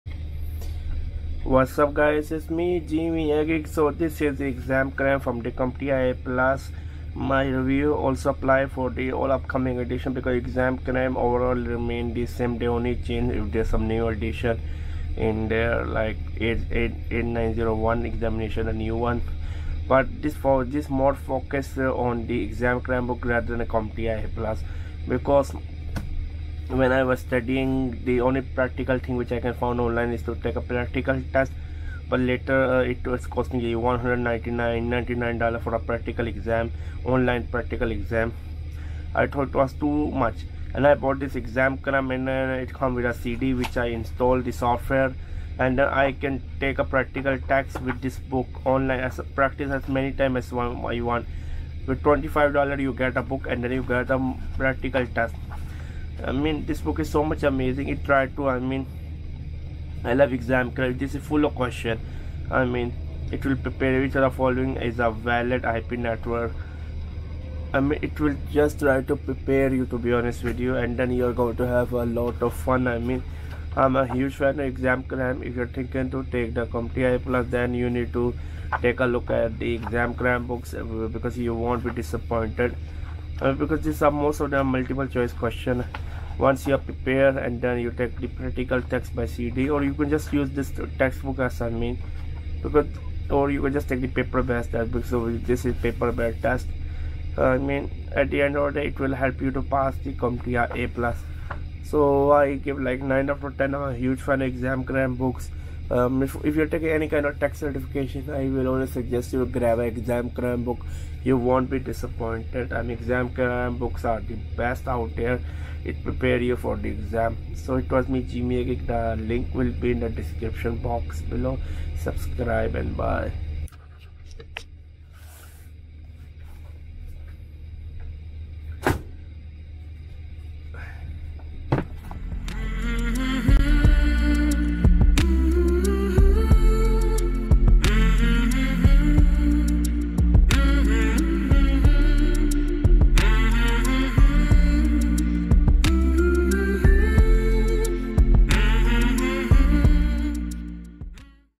CompTIA A+ Exam Cram 📚 Book Review
📚Best Books

CompTIA A+ Exam Cram Review:
🔶Book Name: CompTIA A+ Core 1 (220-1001) and Core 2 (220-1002) Exam Cram
🔷Author: David L. Prowse
🔶Publisher: Pearson IT Certification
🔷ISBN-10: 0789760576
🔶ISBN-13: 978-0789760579

Let's Review the CompTIA A+ Exam Cram Hard Copy and Is this Best Book to Read?